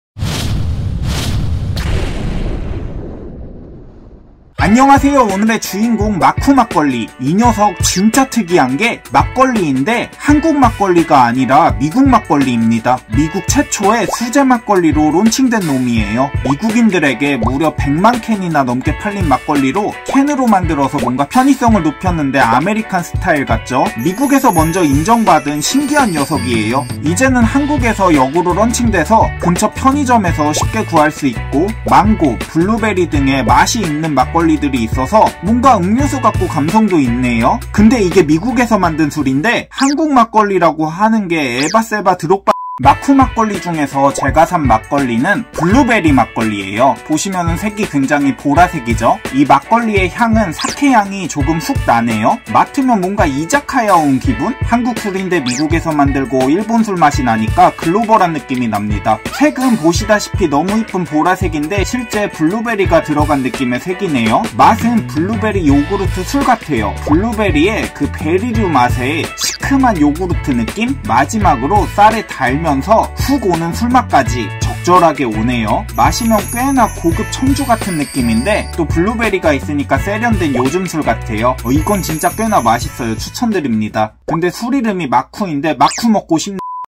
미국에서 100만개 팔린 막걸리? 마쿠 막걸리 (전통주 추천, 전통주 리뷰, 막걸리 추천, 혼술)
본 리뷰는 매우 주관적입니다.
제품명: 마쿠 막걸리
도수: 6도
가격: 약 3천 원 (판매처 별로 다를 수 있습니다.)